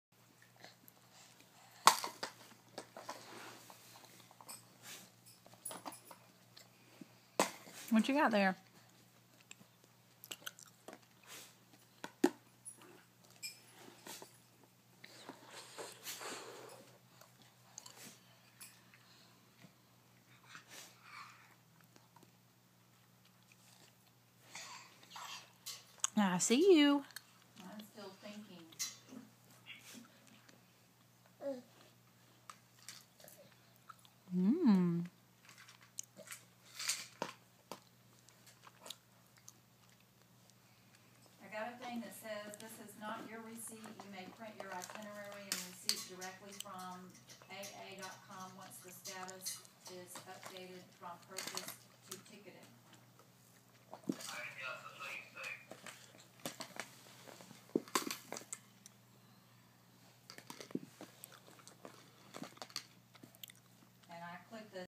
Parker and his stacking rings
Parker plays with stacking rings